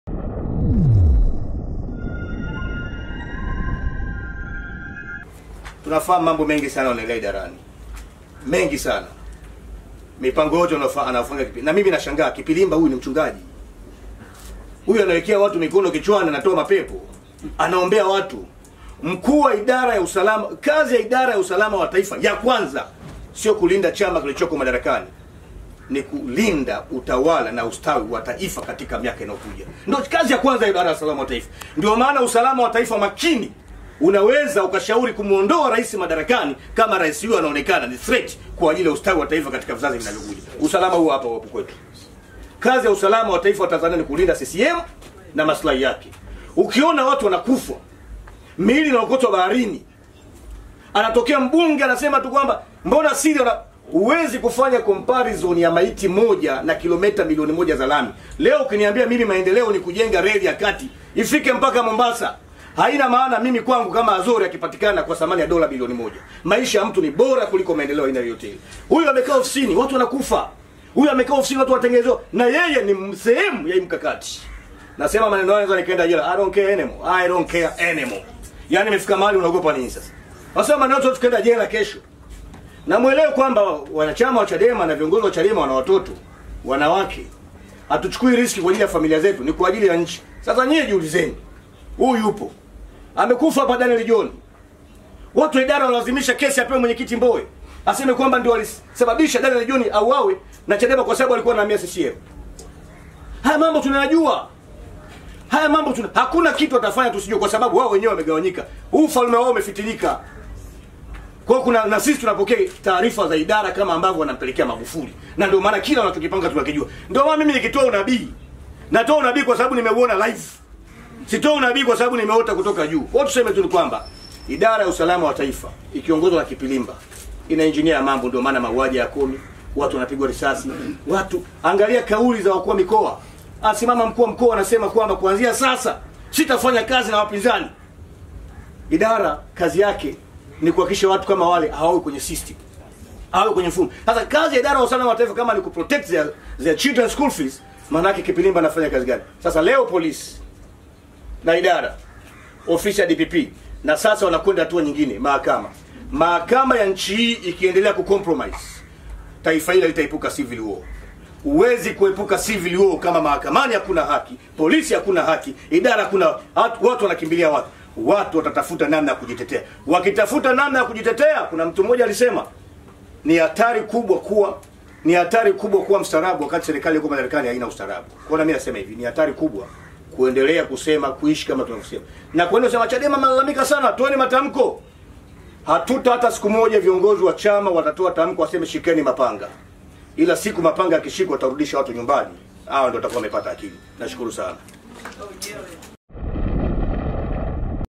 Mhe. Godbless Lema amnyooshe kidole Kapilimba Mkurugenzi wa Usalama wa Taifa
Mhe. Godbless Lema amemtaka Mkurugenzi wa Usalama wa Taifa kuacha kukitetea Chama cha Mapinduzi CCM na badala yake walinde usalama wa Nchi na Mali zake.